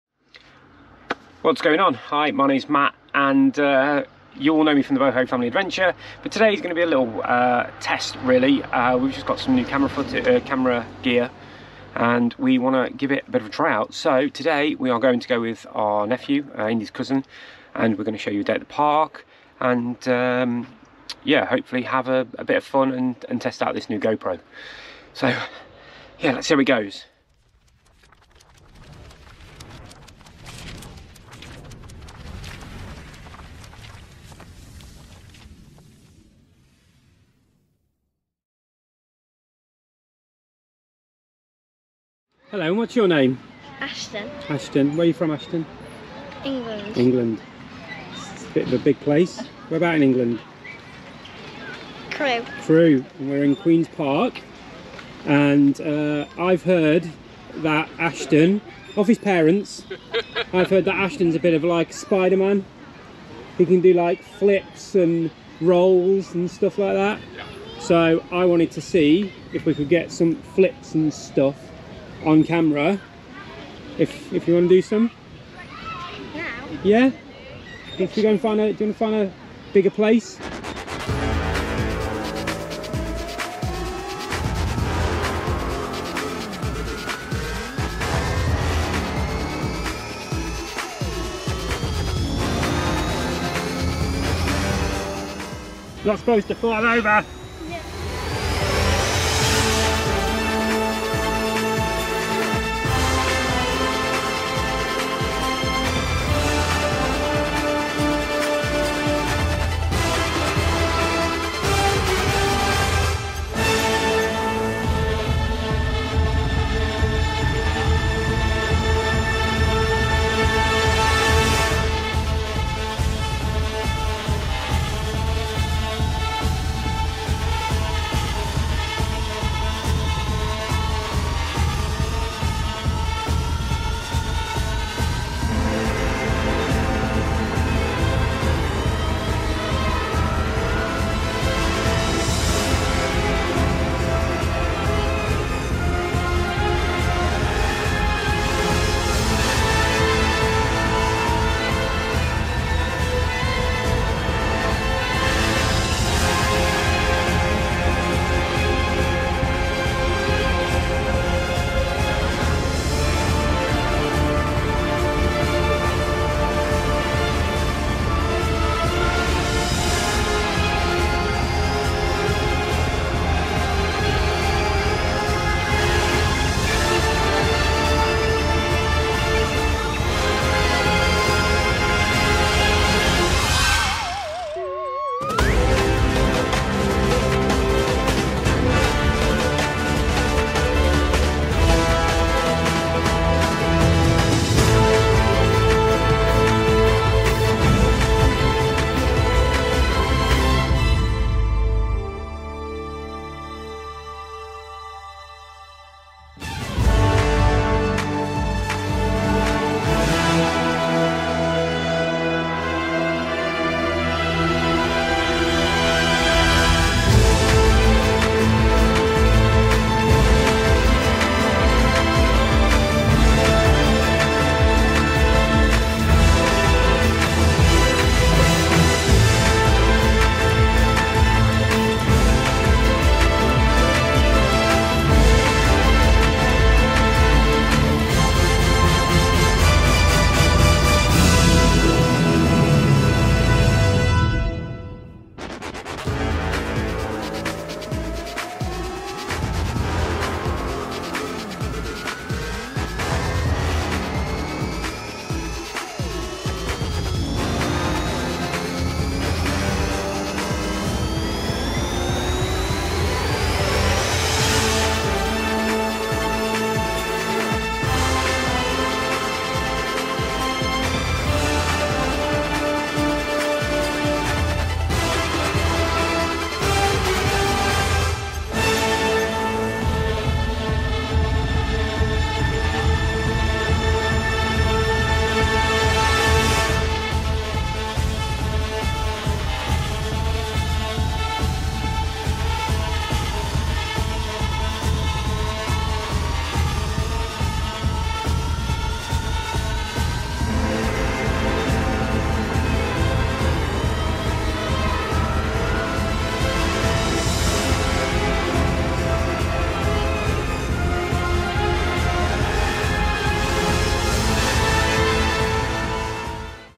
We upgraded our camera equipment.. heres some results
We have just had a bit if a break, but wanted to test out our new camera equipment.

So let me introduce you to Ashton :)